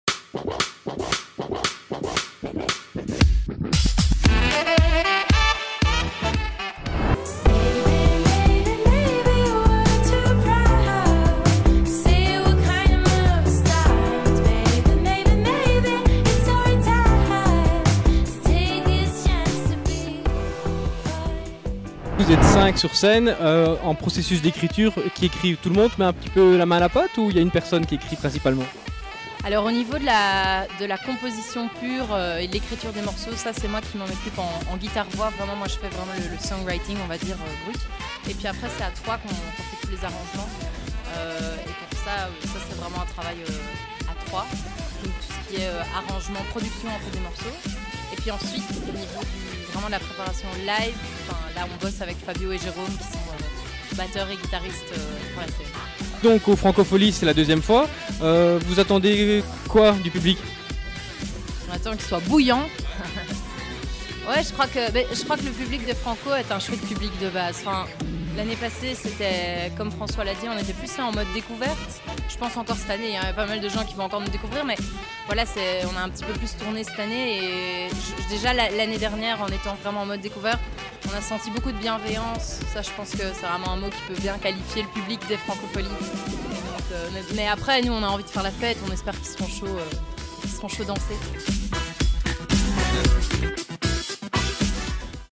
Francofolies de Spa - Sonnfjord
Francofolies de Spa - CHARLEKING RADIO / FullTV

Rencontre avec Sonnfjord présent aux Francofolies de Spa.